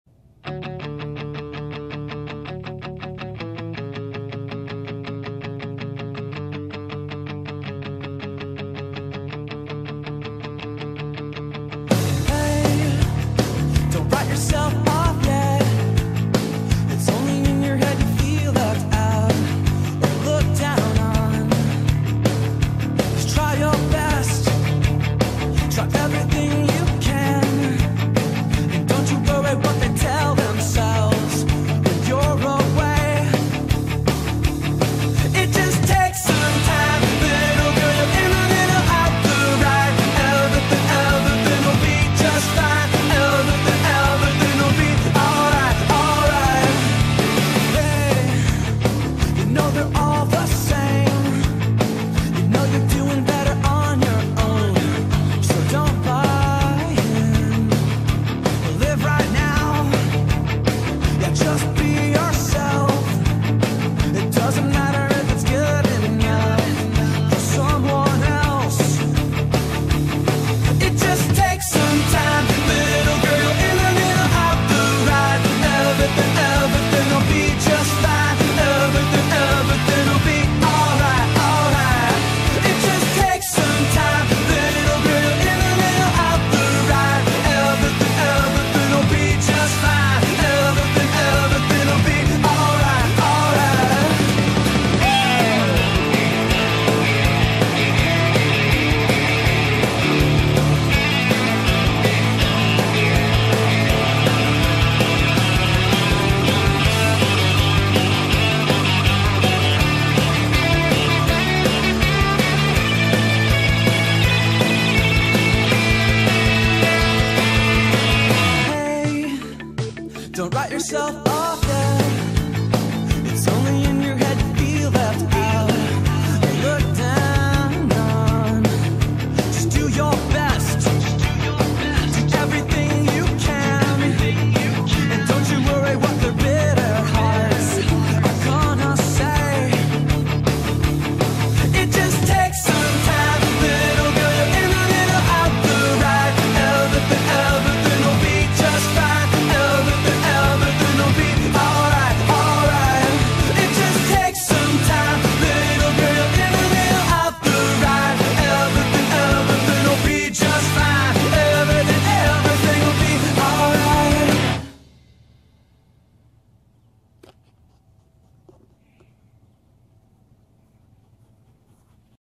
Disc Dog Practice
Bindi and I practicing frisbee